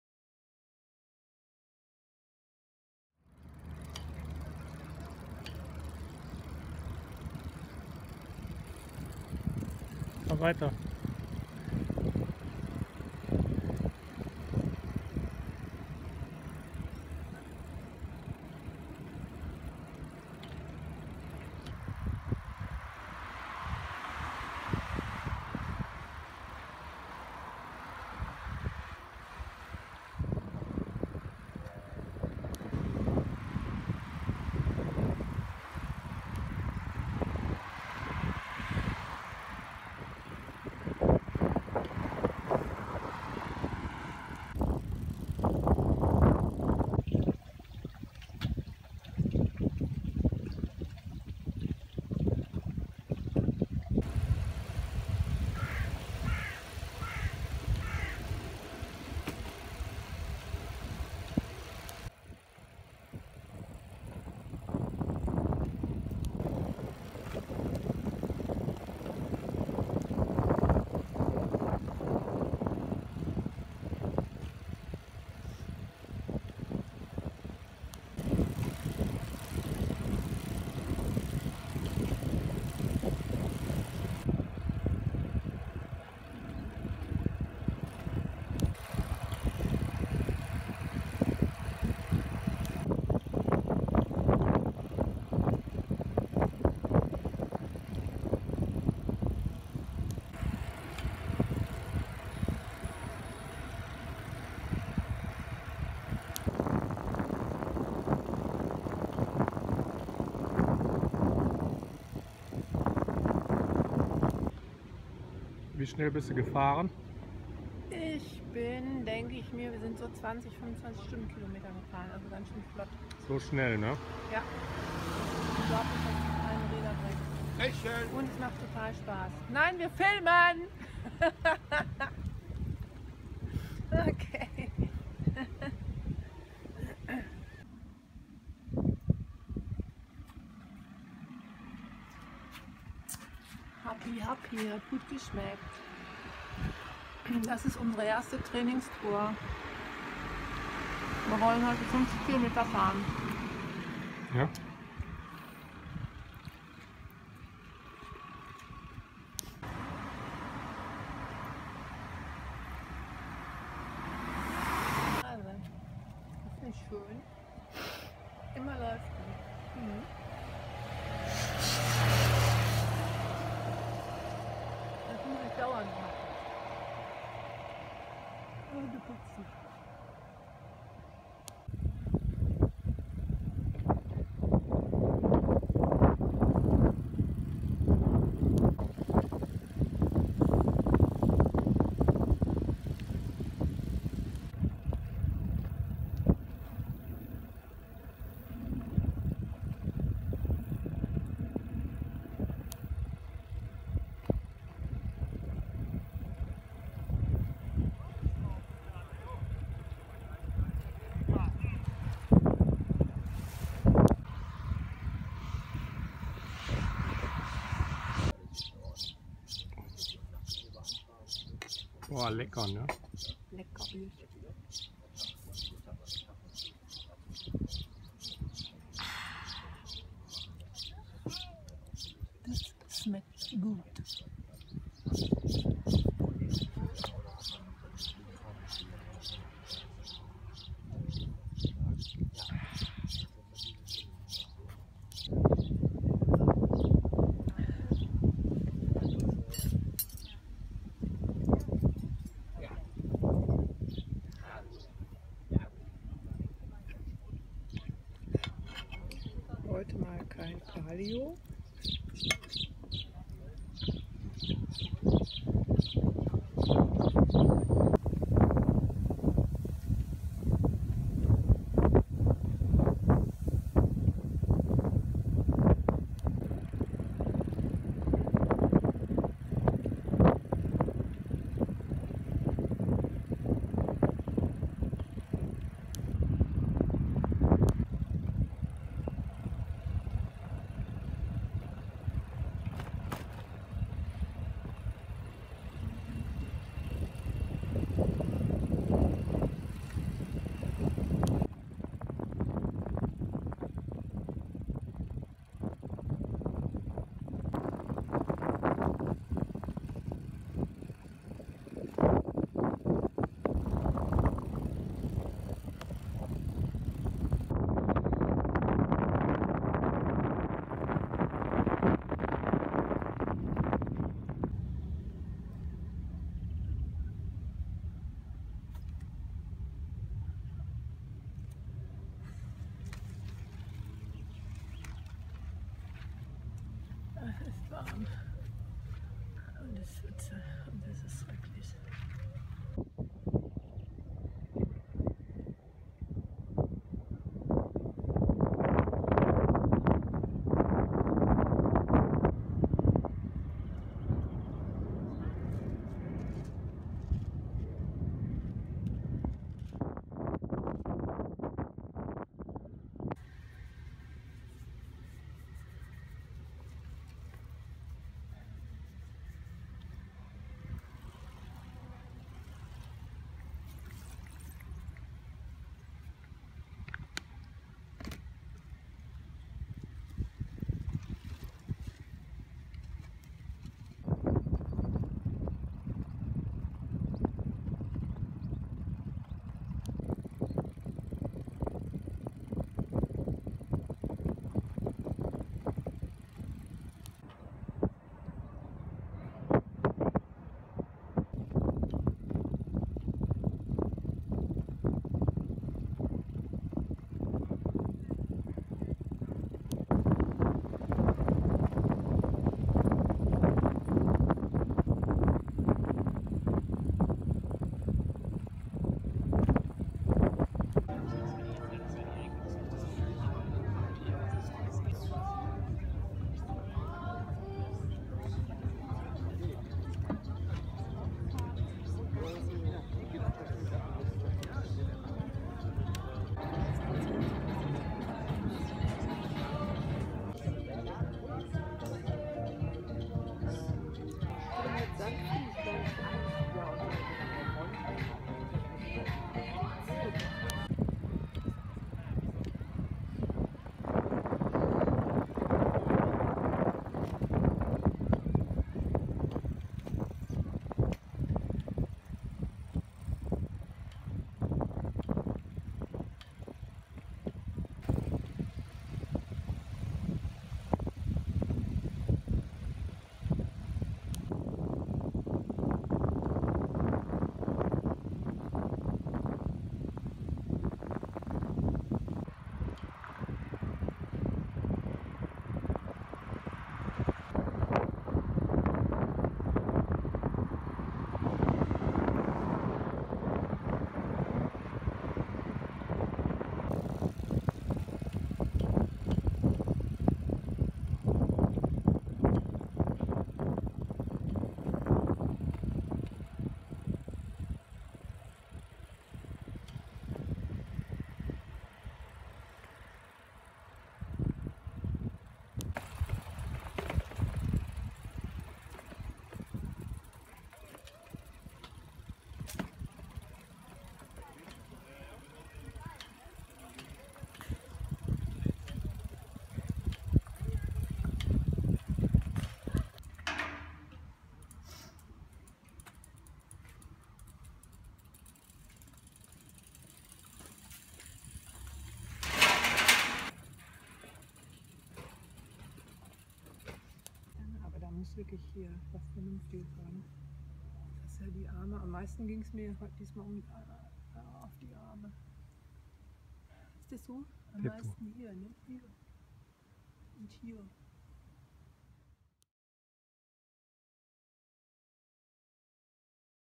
Brompton Tour Duisburg 2016
Short Sunday cycle tour with our Bromptons along the Rhine at Düsseldorf and Duisburg